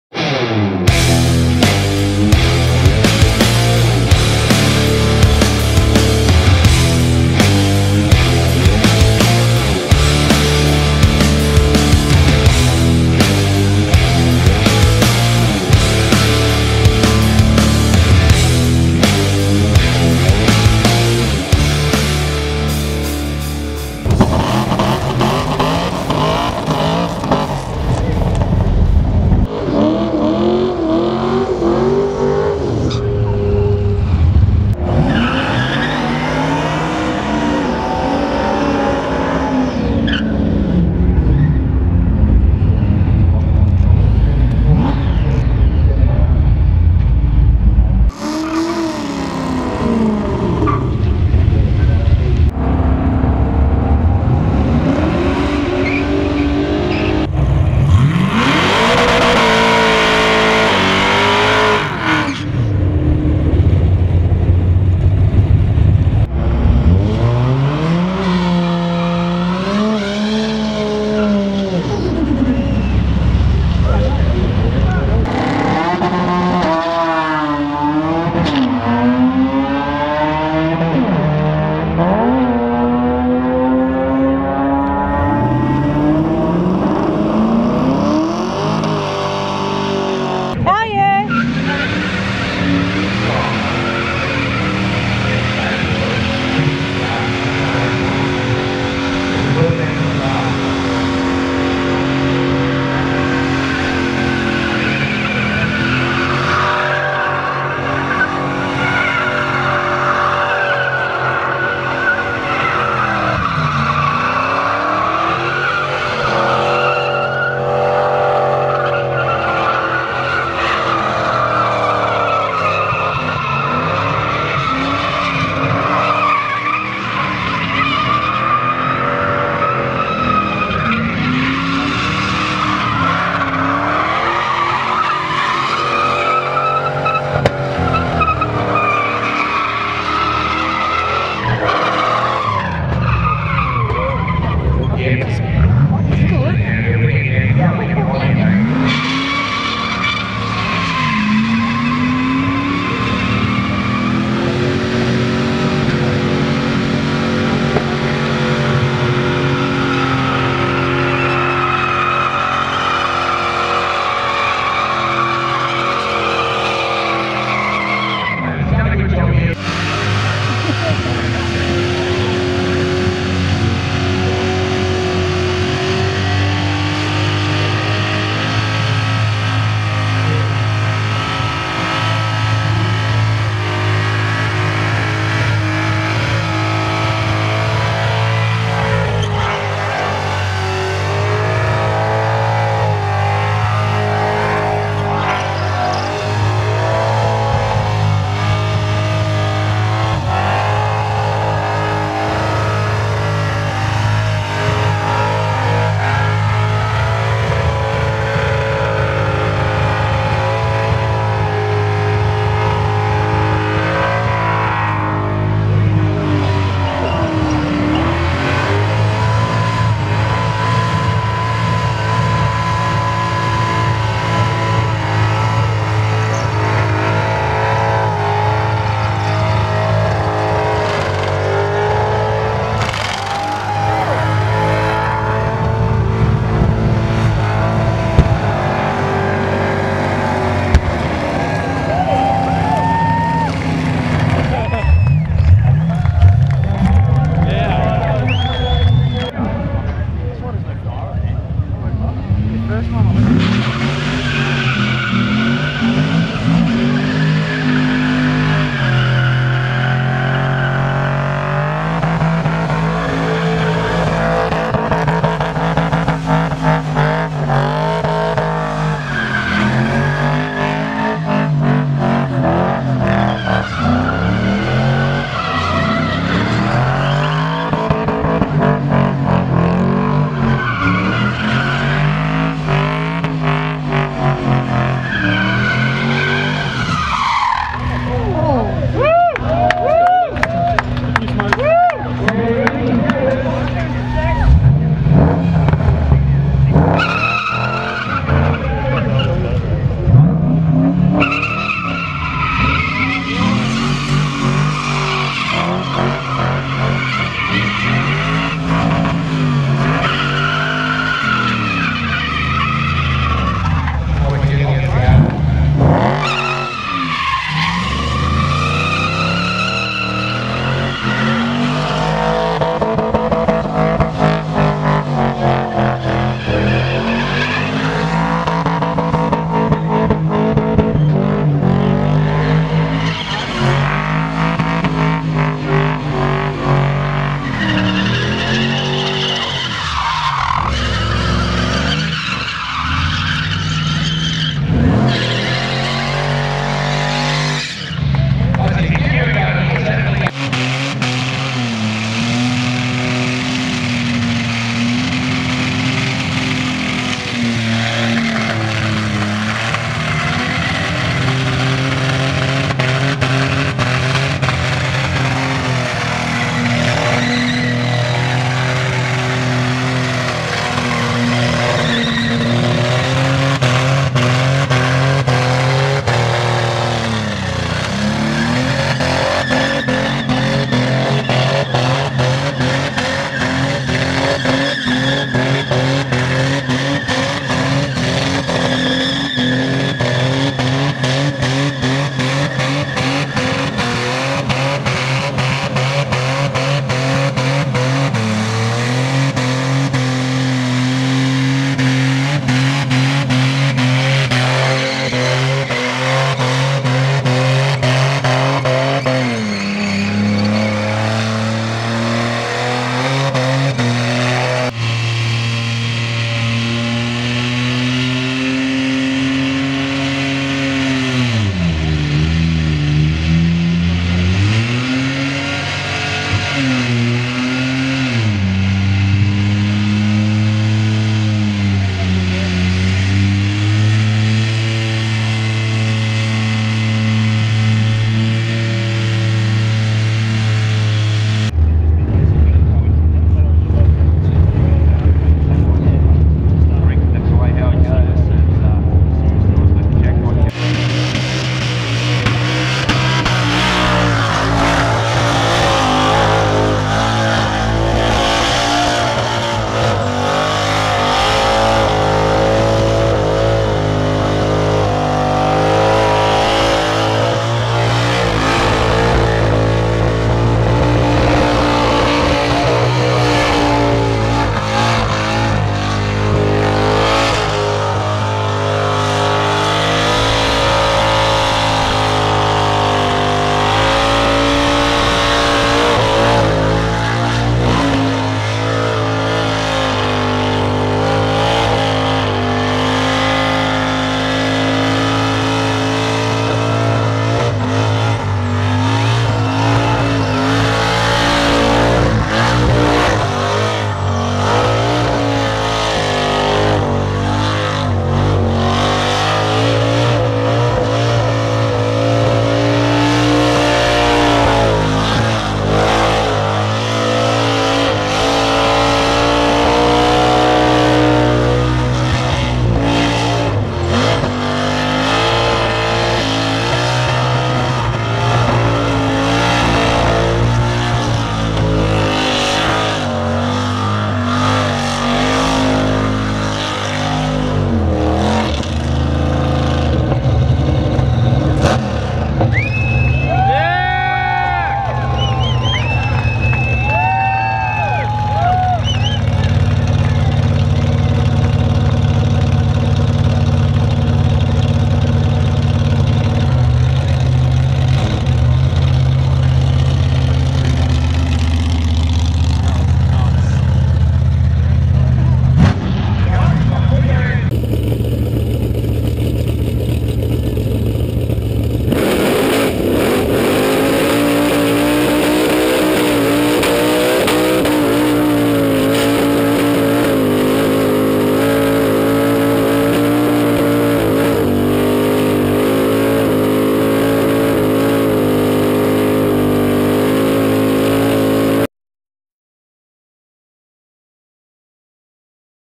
WSID 14-9-11 Burnout Compilation
From Hi Tec Oils Race 4 Real wednesday nights, here is compilation of pretty much just burnouts. Enjoy.